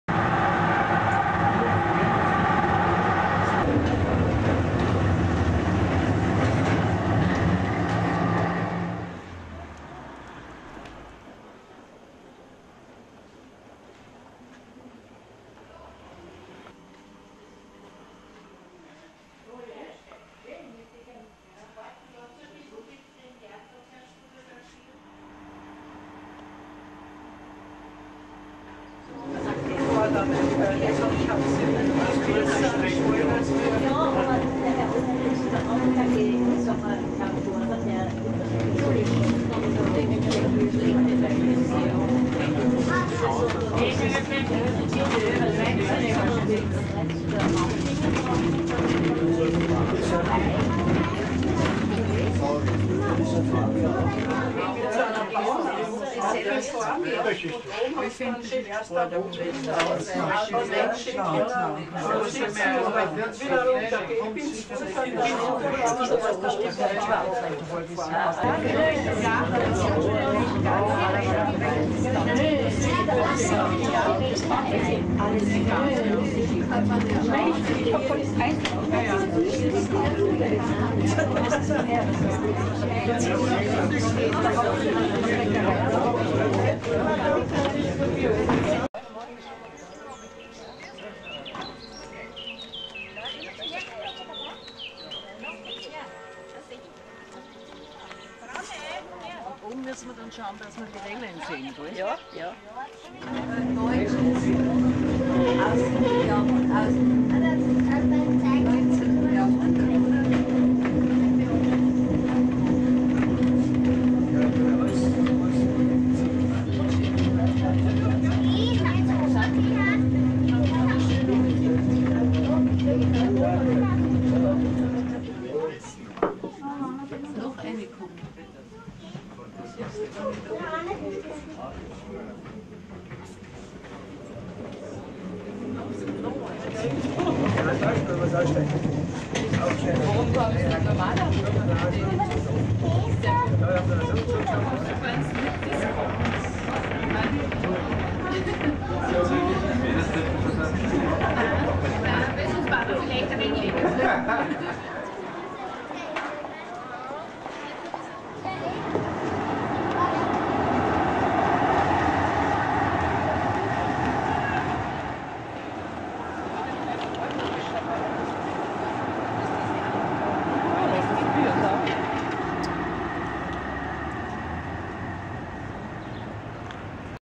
giro con il tram della Pöstlingerbahn, nel 1998 / Linz Pöstlingbergbahn
Giro con il tram della Pöstlingerbahn. Nel maggio 1998, prima dell'ammodernamento dei veicoli,  del cambio di scartamento, e del prolungamento, era l'unico tram al mondo con le ruote a bordino esterno.

A 00.28 minuti si vede la particolare forma delle rotaie, e a 1.00 minuti si vede un deviatoio con l'ago esterno al binario!
Pöstlingerbahn Straßenbahnfahrt. Im Mai 1998, vor der Modernisierung der Fahrzeuge, dem Spurwechsel und der Erweiterung, war es die einzige Straßenbahn der Welt mit Rädern mit einer Außenkante. Bei 00.28 Minuten kann man die besondere Form der Schienen sehen, und bei 1.00 Minuten sieht man einen Deviator mit der Nadel außerhalb der Bahn!